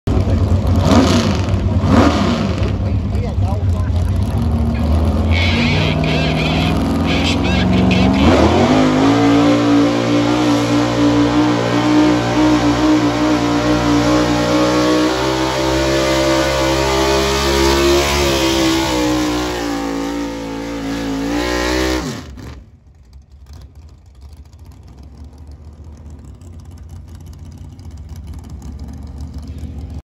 Super Stock 4WD Class At Owenton Ky Fall Brawl 2020
Fall Brawl 2020 Owenton Ky